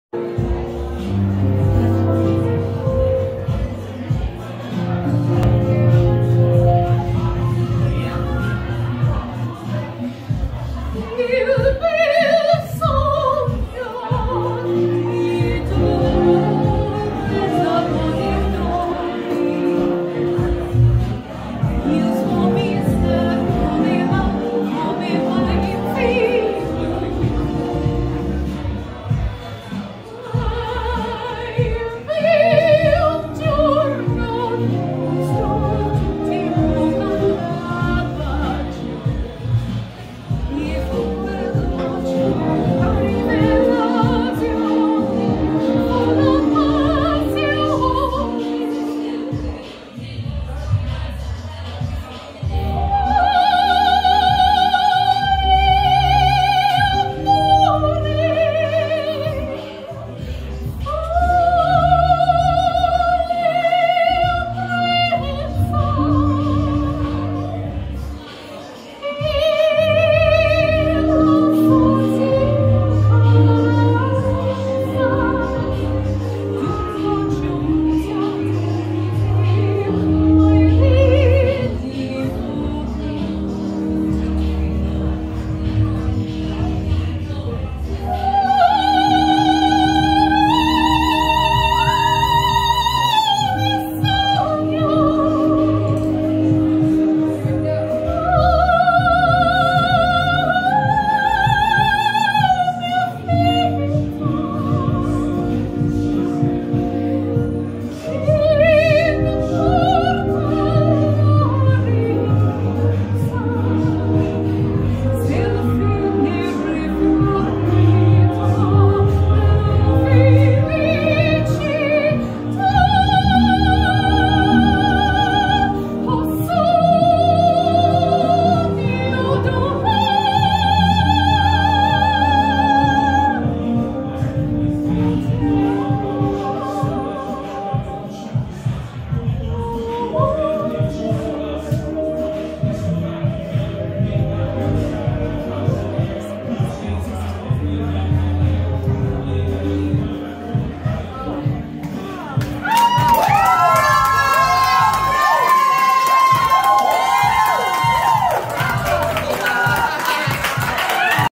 Performing
Baroque Spring Fling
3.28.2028
A collaboration with @ms.yoo and @mzkittykitty_dj24
Featuring
doriansgildedsociety Doriansgildedsociety. org
Did you miss this one? Well, come to the next one. April 25th, 2024. See you there.